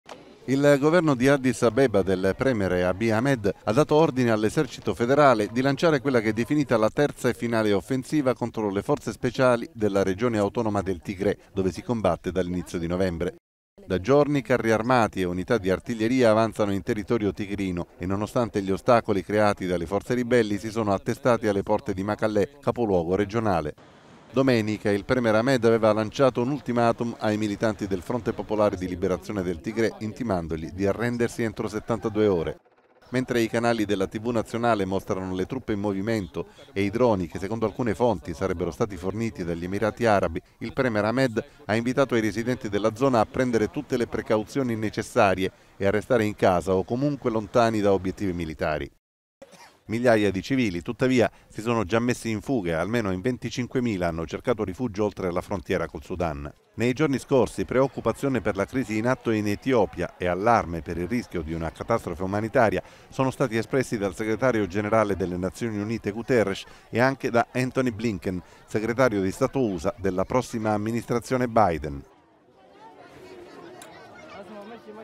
Ahmed ordina: "Offensiva finale nel Tigrè"
TImori di un bagno di sangue. Il premier etiope invita i residenti a "restare lontani dagli obiettivi militari". Finora 25.000 profughi rifugiati in Sudan